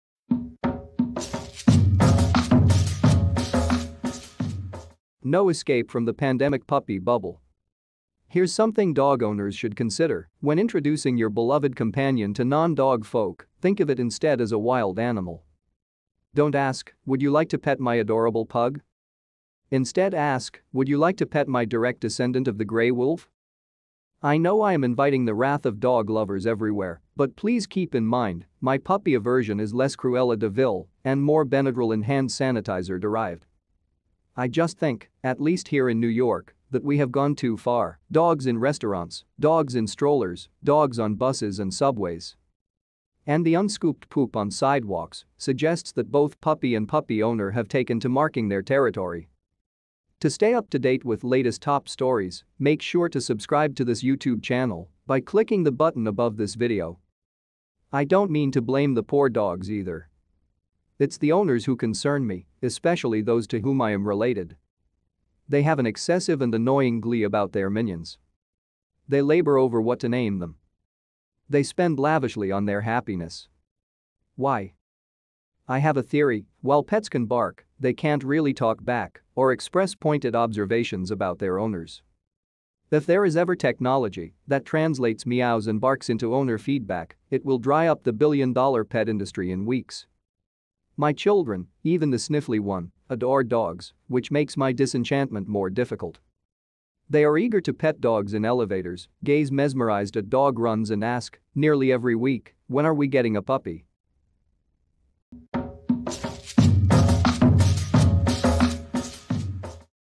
No Escape From the Pandemic Puppy Bubble
Here’s something dog owners should consider: When introducing your beloved companion to non-dog folk, think of it instead as a wild animal.
Don’t ask: Would you like to pet my adorable pug?
Instead ask: Would you like to pet my direct descendant of the gray wolf?
I know I am inviting the wrath of dog lovers everywhere, but please keep in mind, my puppy aversion is less Cruella de Vil and more Benadryl and hand-sanitizer derived.
I just think, at least here in New York, that we have gone too far: dogs in restaurants, dogs in strollers, dogs on buses and subways.
And the unscooped poop on sidewalks suggests that both puppy and puppy owner have taken to marking their territory.
I don’t mean to blame the poor dogs either.
It’s the owners who concern me, especially those to whom I am related.
They have an excessive and annoying glee about their minions.